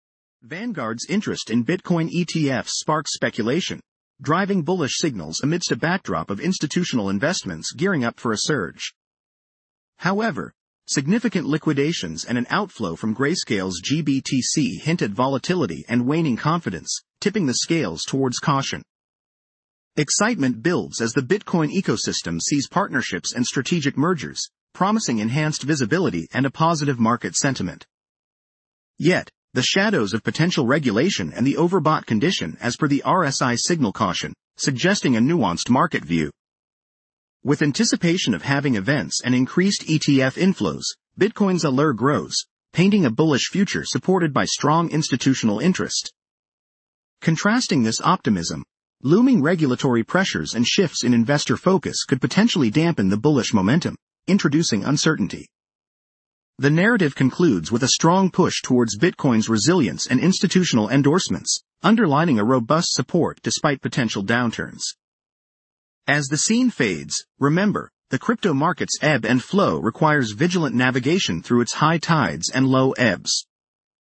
Bitcoin's Wild Ride: Bull vs Bear, Who Wins?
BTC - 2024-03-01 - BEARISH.

A quick overview of the latest Bitcoin micro signals:
- Major investments and ETF interest suggest bullish trends.
- Liquidations and regulatory news hint at potential volatility.
- Institutional movements influence Bitcoin's market direction.